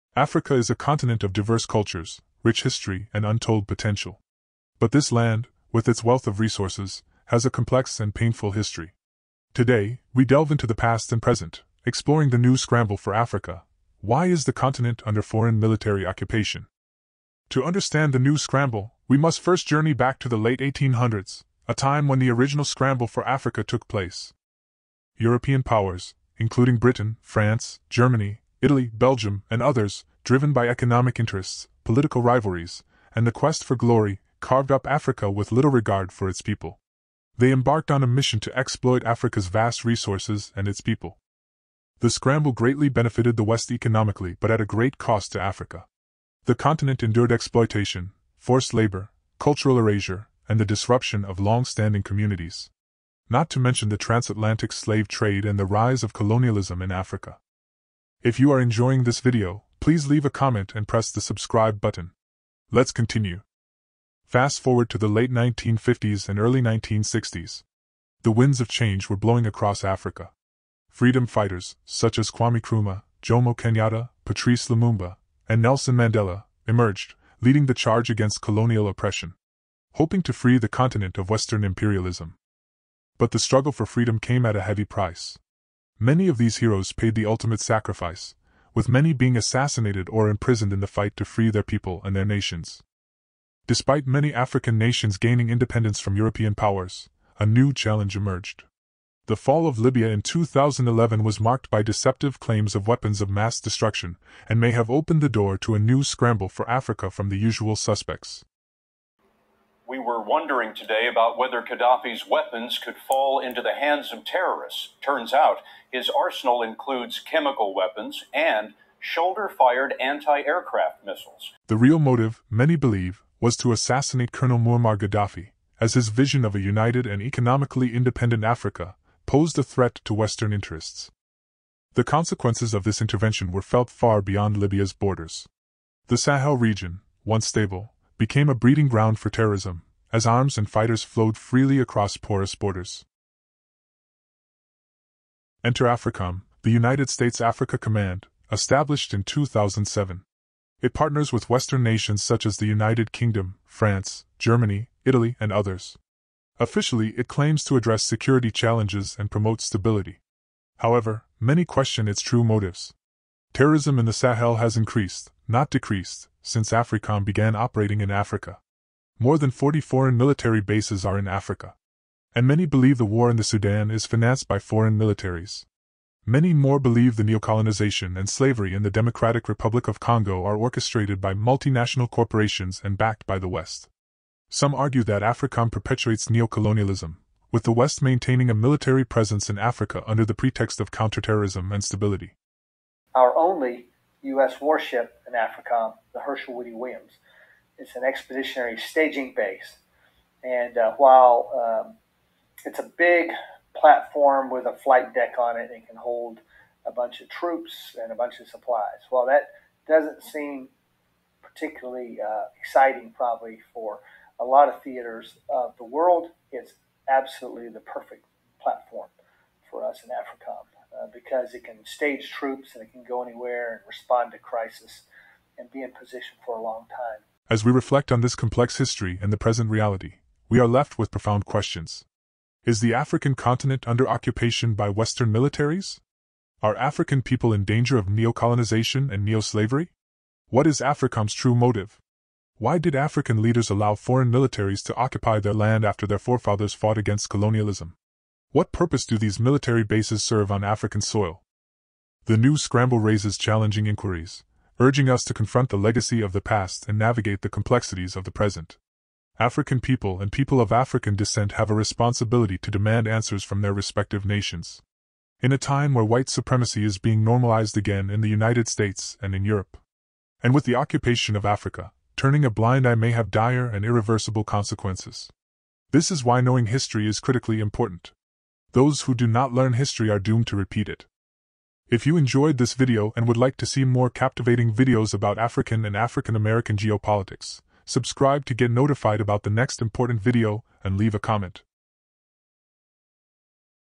The New Scramble for Africa: Is Africa under Western Military Occupation?
More than 40 Western military bases are in the continent of Africa. What does this mean to Africans, African Americans, and people of the African diaspora?

African History
African American History
Scramble for Africa
African Geopolitics
Military Occupation
African American Geopolitics
White Supremacy
Western military bases
black history